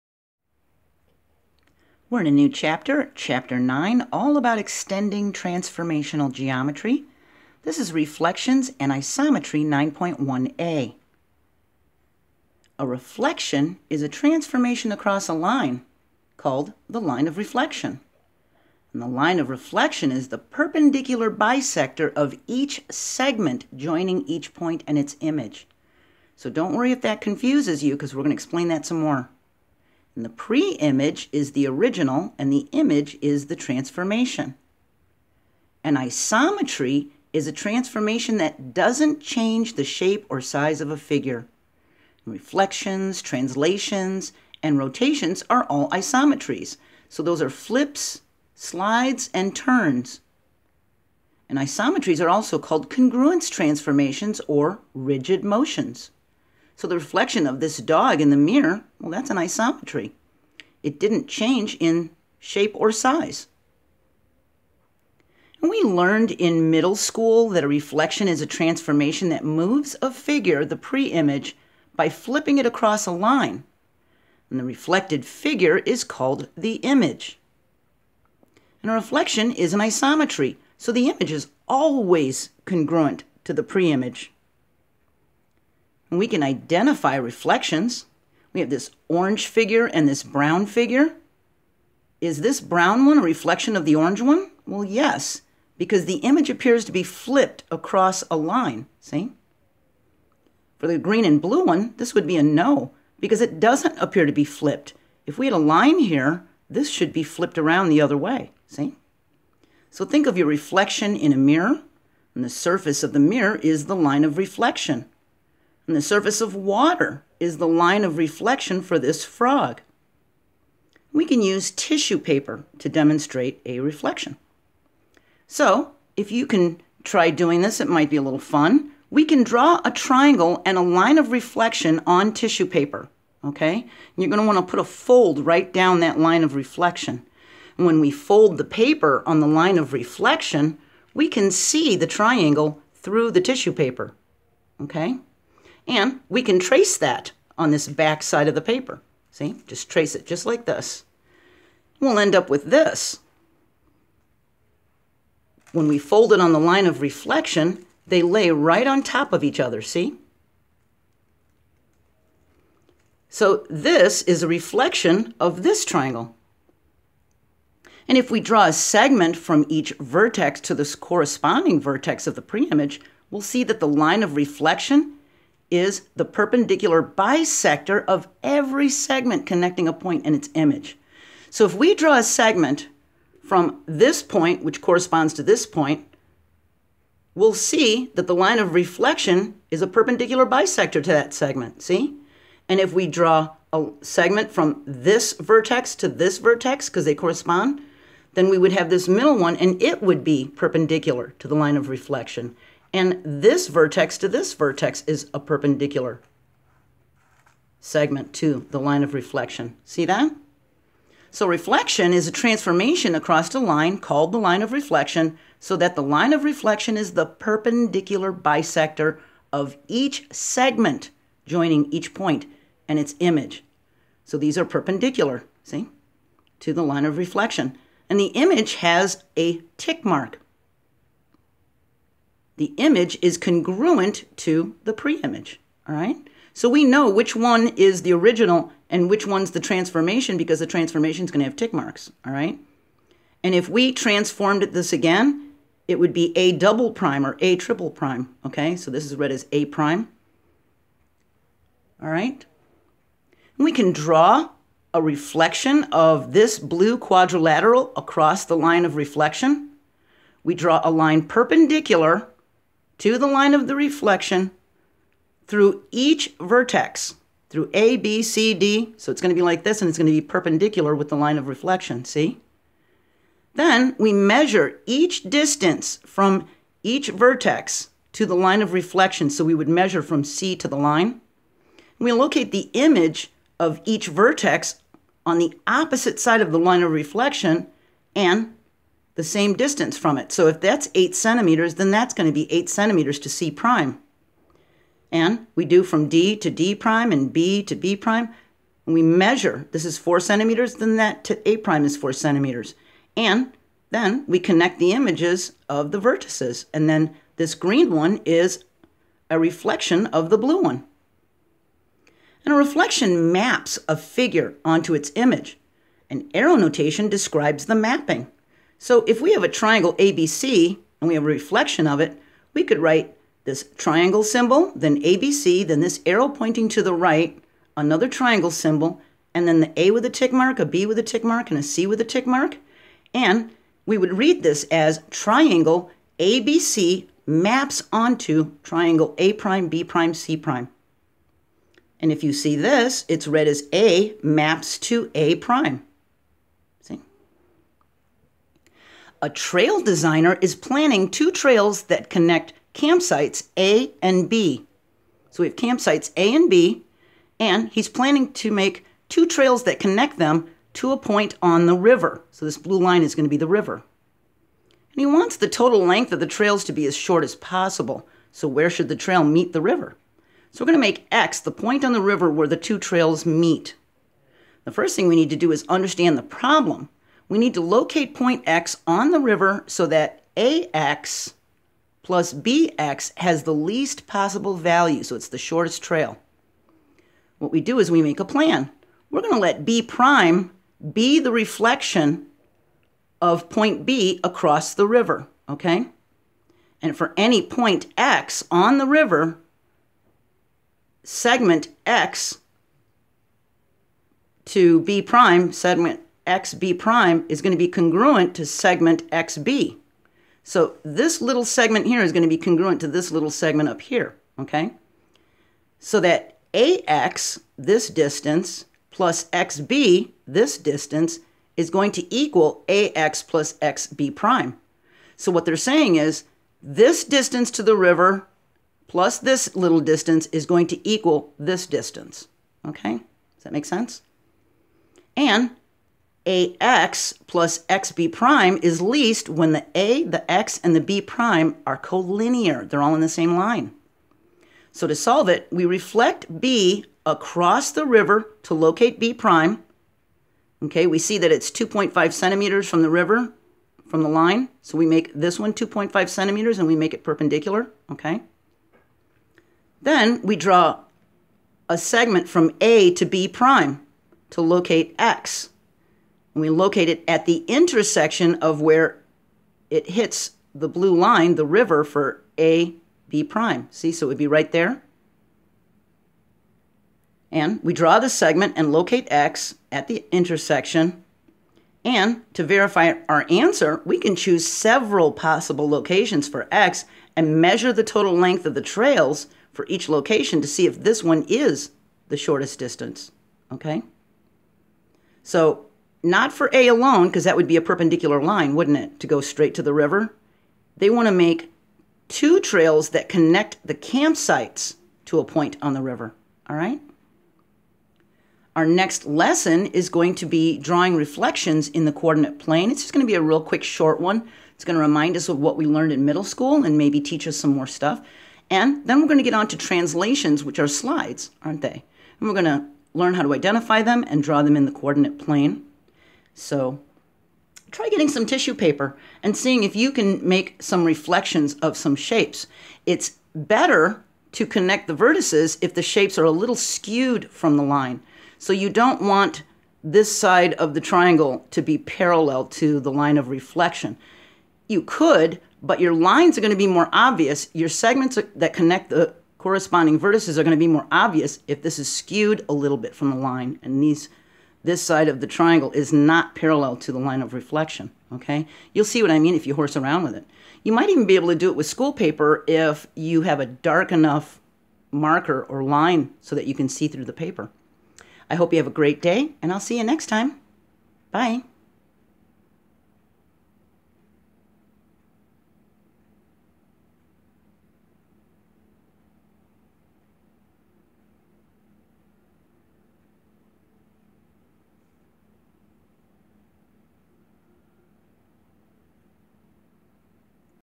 Geometry 9.1a, Reflections & isometry
An explanation of how a reflection is a transformation across a line called the "Line of Reflection". How the line of reflection is the perpendicular bisector of each segment joining each point and its image. An isometry is a transformation that doesn't change the shape or size of a figure, such as a reflection, translation, or rotation. We identify reflections and we demonstrate a reflection using tissue paper. We draw a reflection of a quadrilateral using perpendicular lines from each vertex of the pre-image. We discuss reflection notation and how to read and write it with tick marks. We find the shortest trail possible from two campsites to a river using a reflection.

MINDS Minds.com/joannsschool